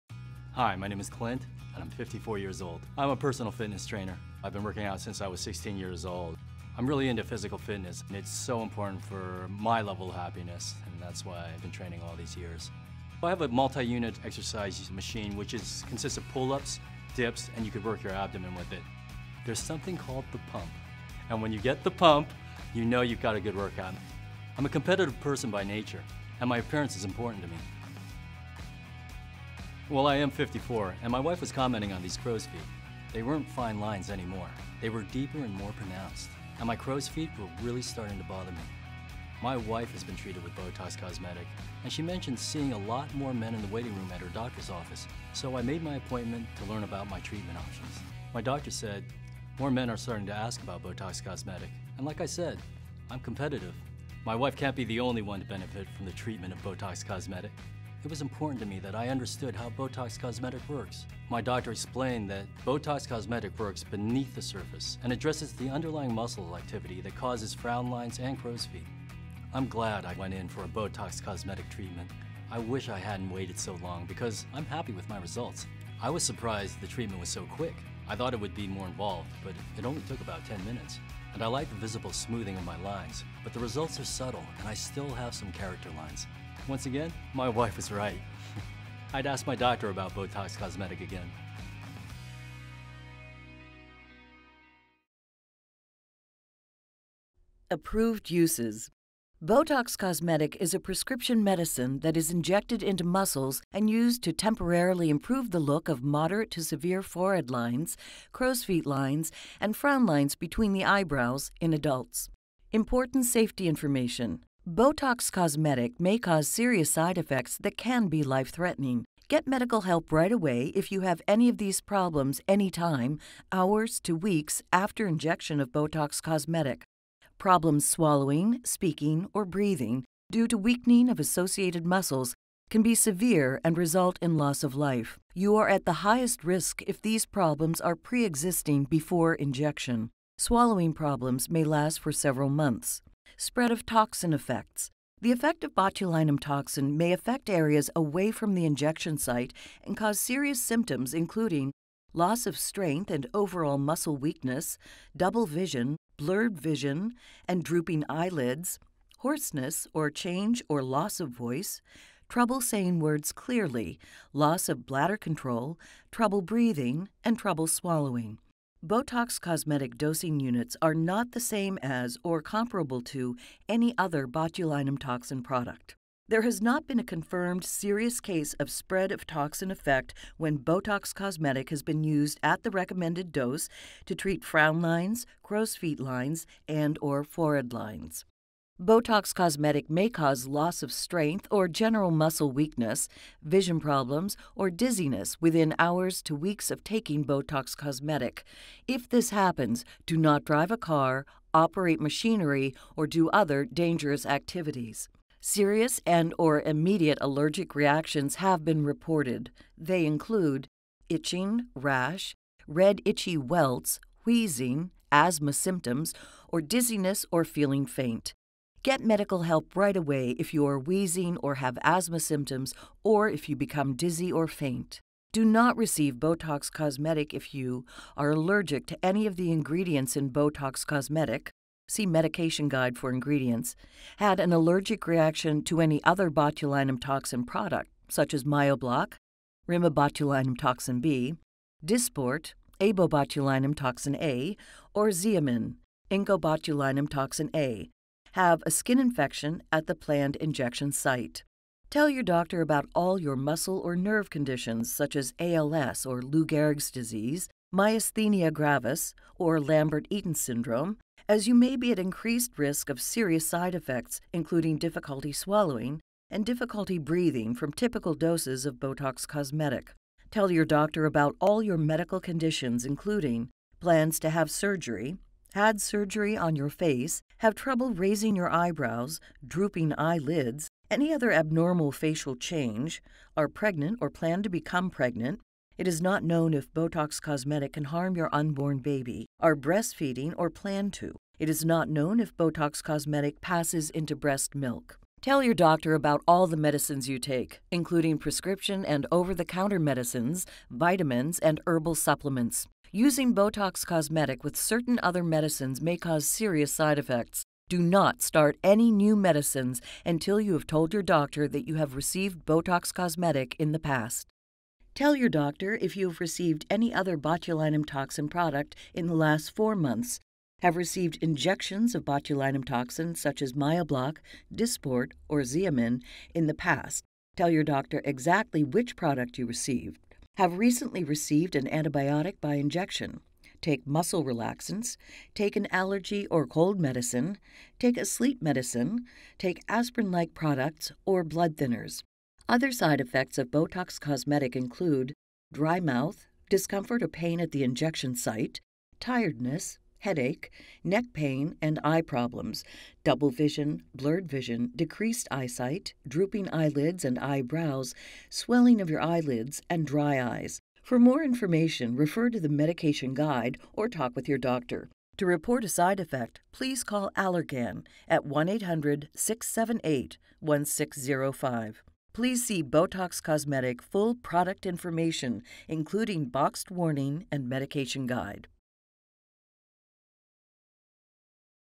Botox "Clint"
Learn more about Clint's journey with Botox and how it gave him the confidence booster that he's been searching for!